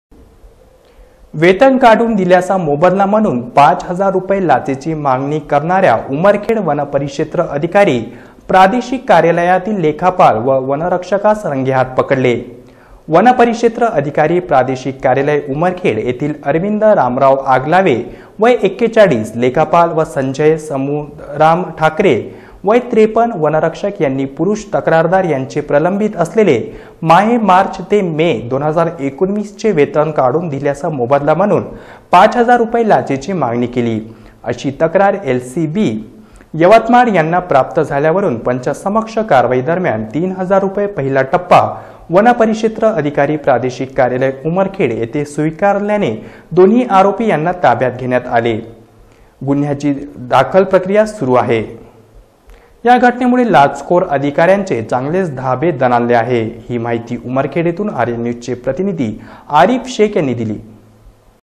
RN NEWS उमरखेड़ येथील लेखपाल व वनरक्षक एसीबीच्या जाळयात.
उमरखेड़ येथील लेखपाल व वनरक्षक एसीबीच्या जाळयात.